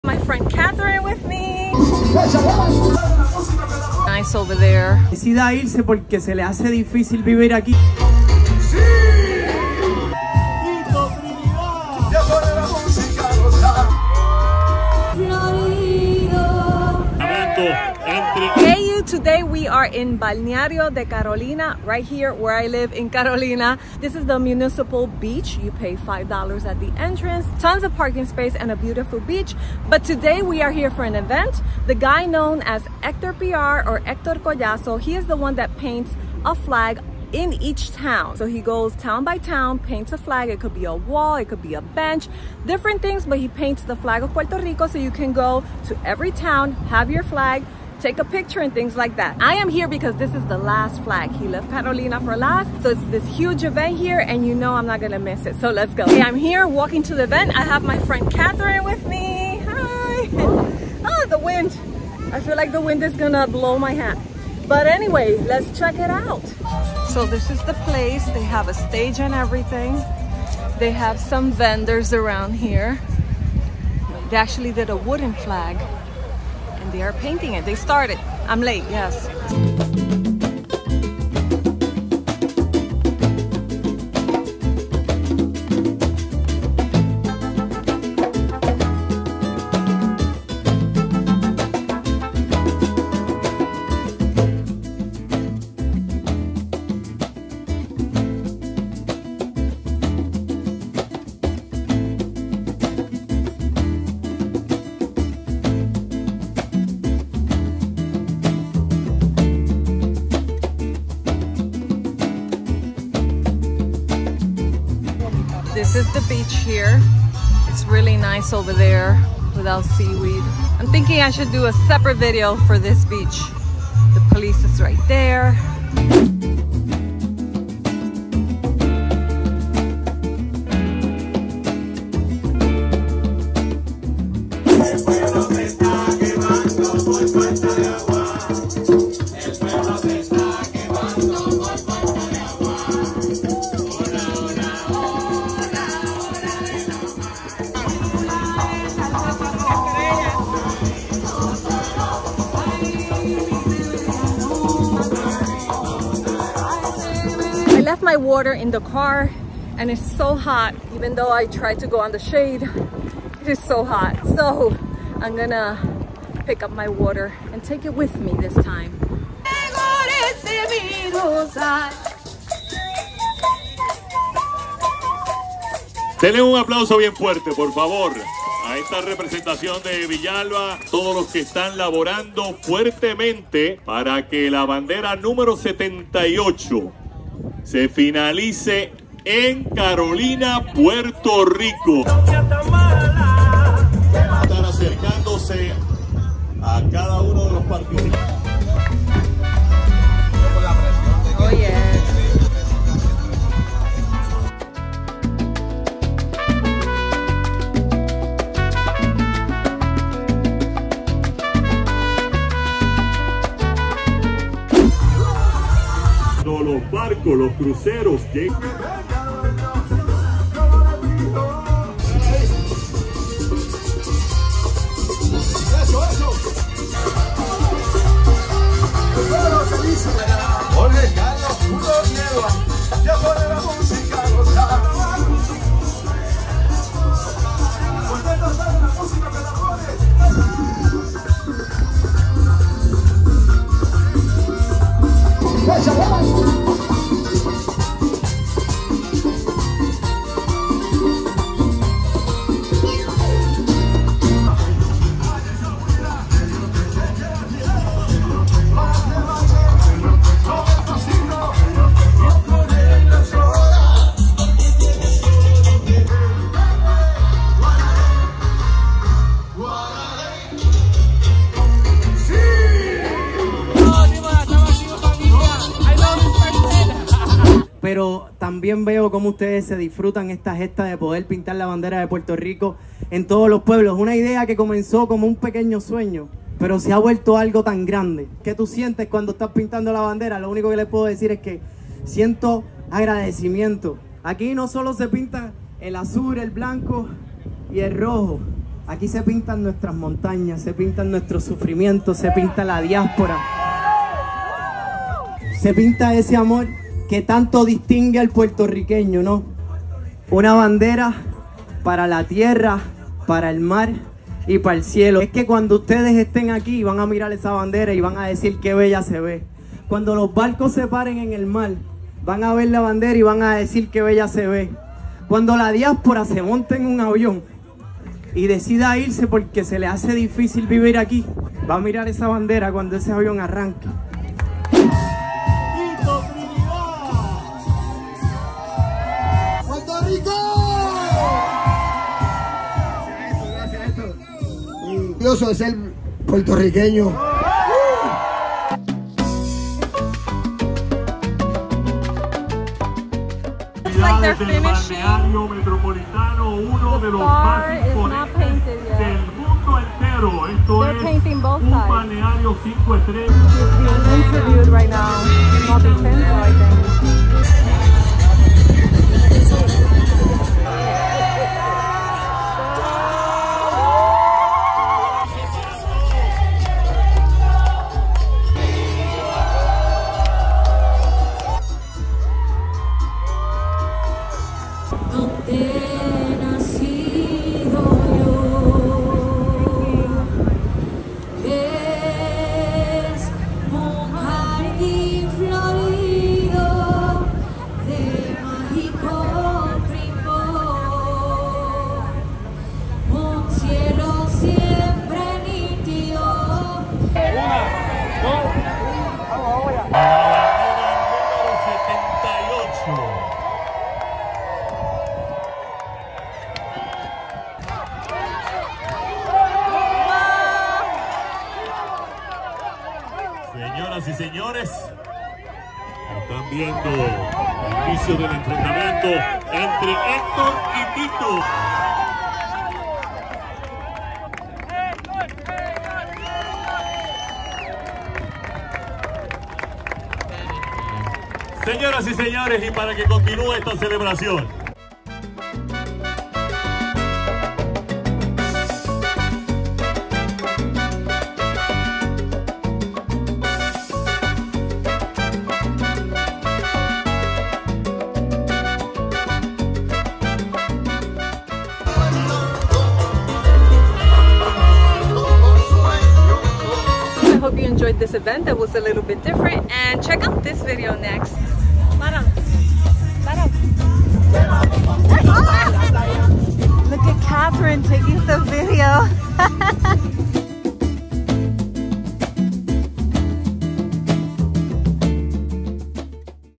Last flag of Puerto Rico is in my town of Carolina! (Bandera #78)
Join me at this unique event celebrating the last flag painted, that is part of the project called “78 pueblos y 1 bandera” by artist Hector PR. This is flag #78 and it’s non other than in the town I currently reside in and specifically located in the Balneario de Carolina. You can also enjoy the different festivities, view of the beach, and special guest Tito Trinidad.

Filming equipment:
Mini desktop tripod/selfie stick
Neewer Smartphone Video Rig, Filmmaking Cage
Rode Mic
Mac Laptop
Portable light

AMAZON STORE FRONT